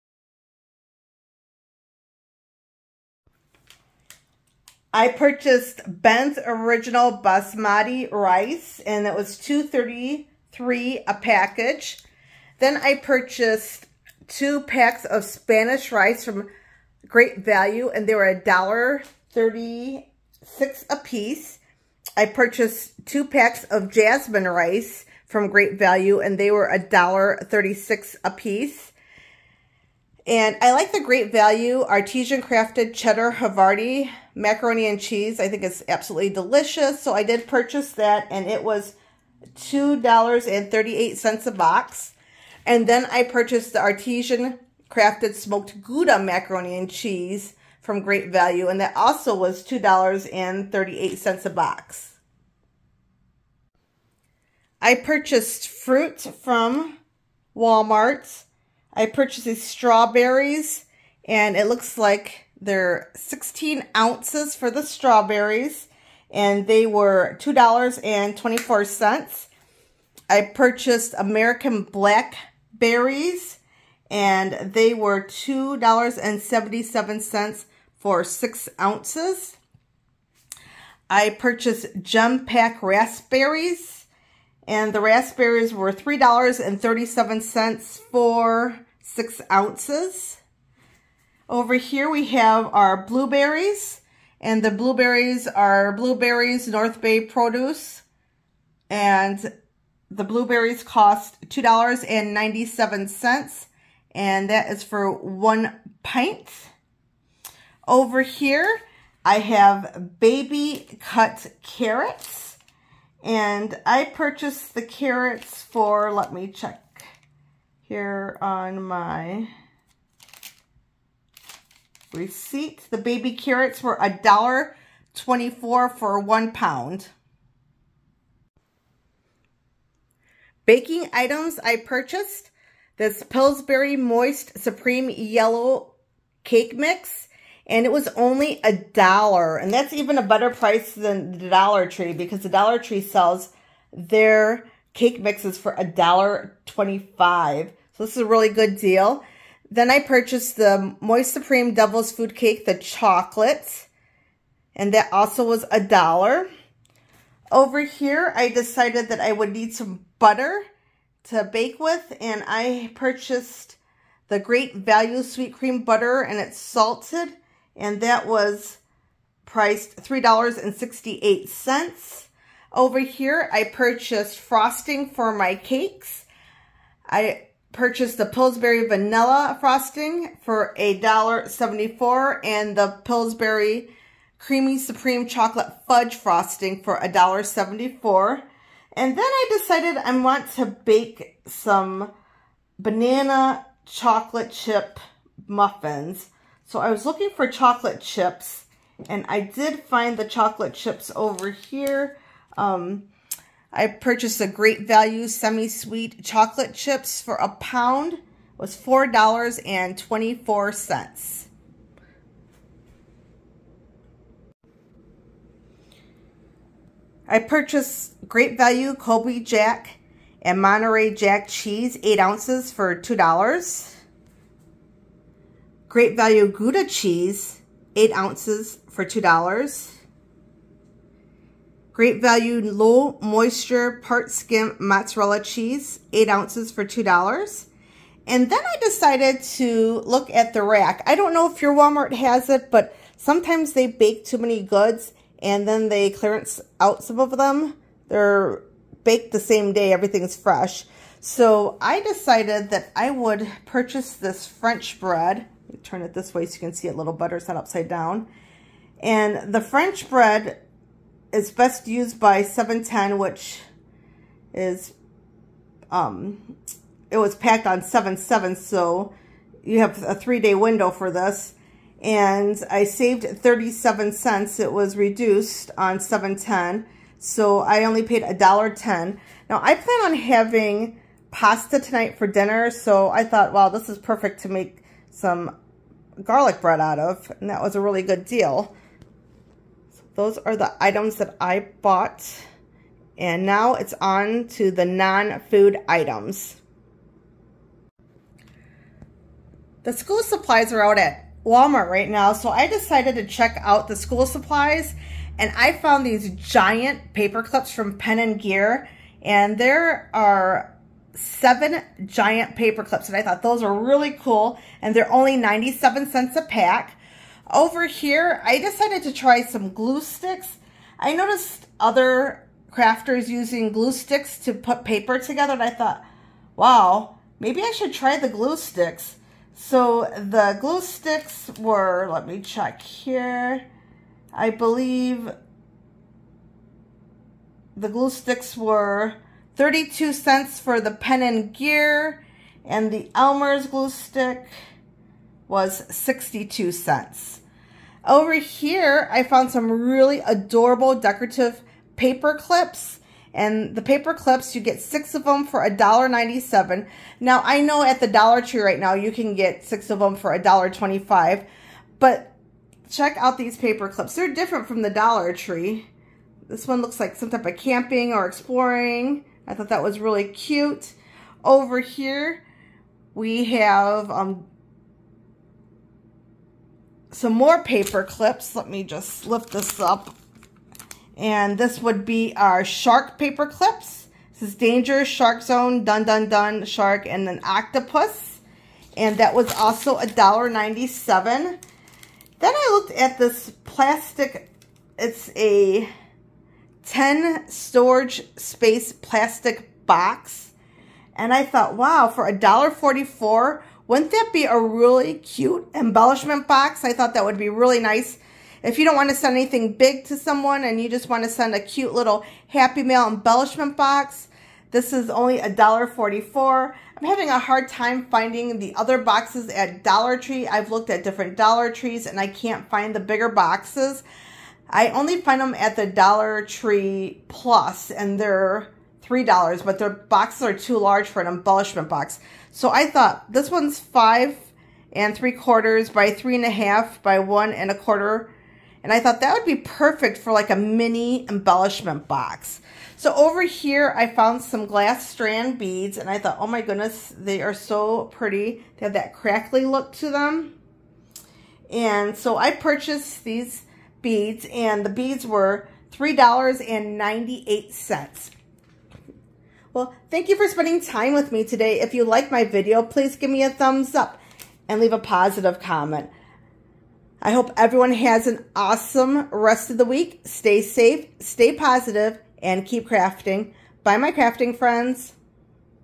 What did I buy at Walmart?
Mini grocery trip to Walmart. What did I buy?